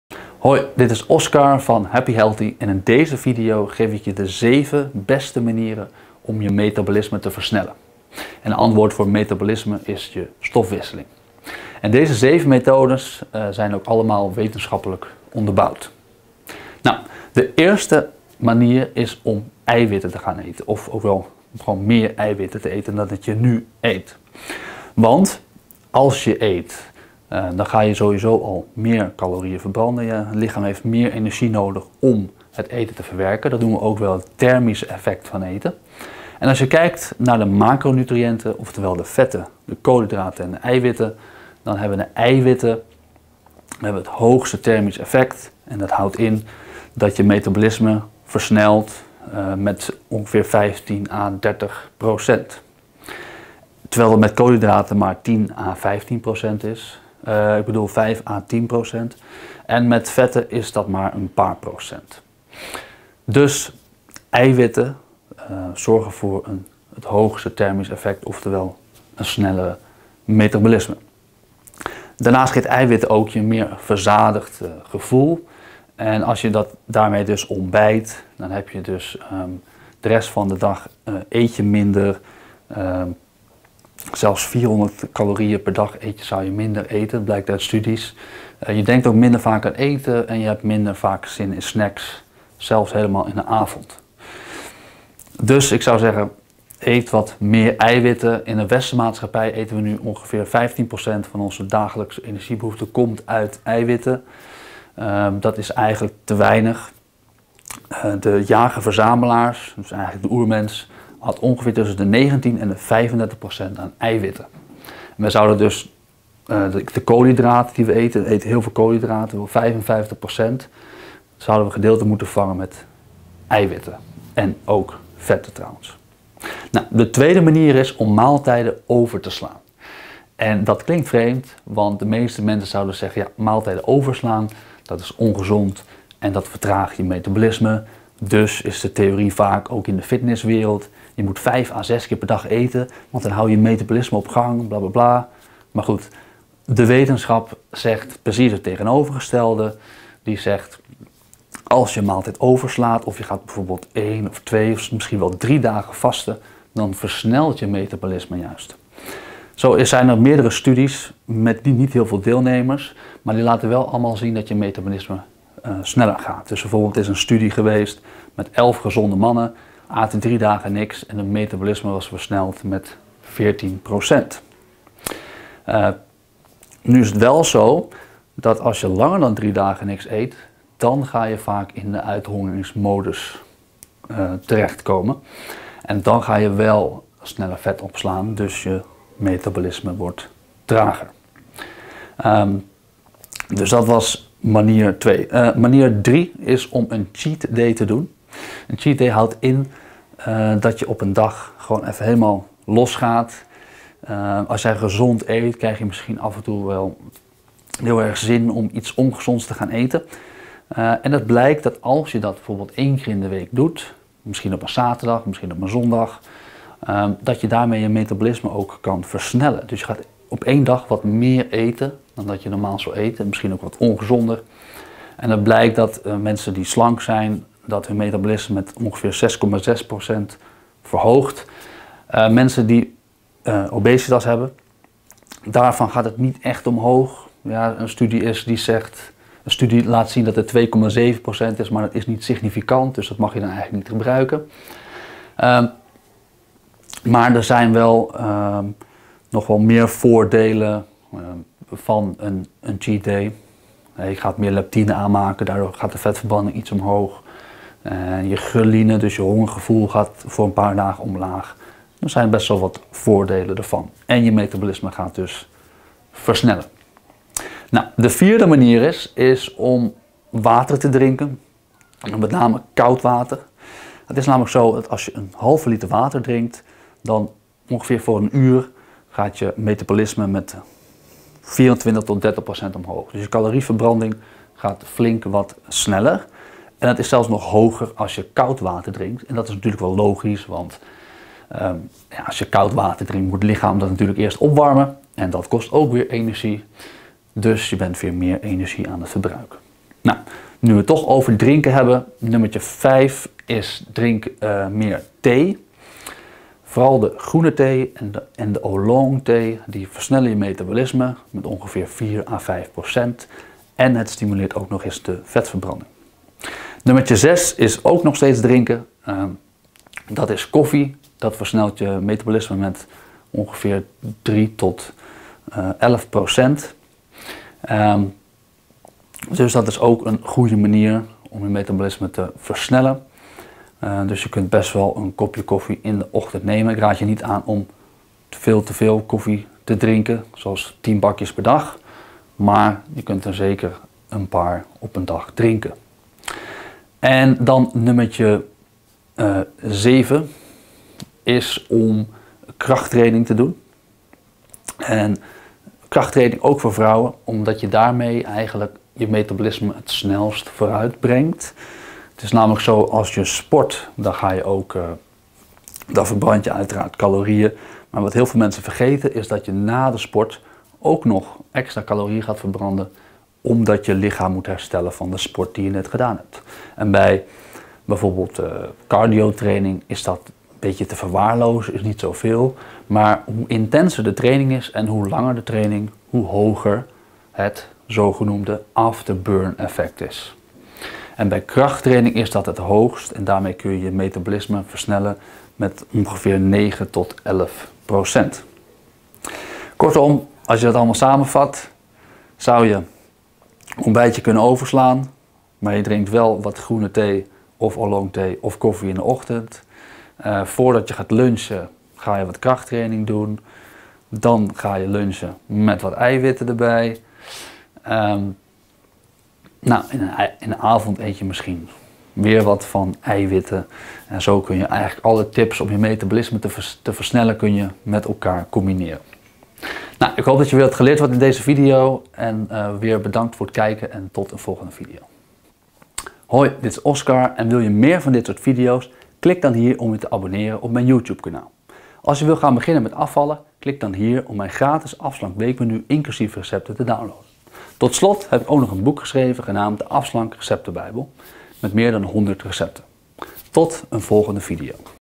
7 Beste Manieren om je Metabolisme te Versnellen
Manier #1: eet (meer) eiwitten
Manier #2: sla maaltijden over
Manier #3: doe aan een cheatday!
Manier #4: drink meer (koud) water
Manier #5: drink thee
Manier #6: drink koffie
Manier #7: doe aan krachttraining (ook voor vrouwen!)

Groet,
Oscar Helm
HappyHealthy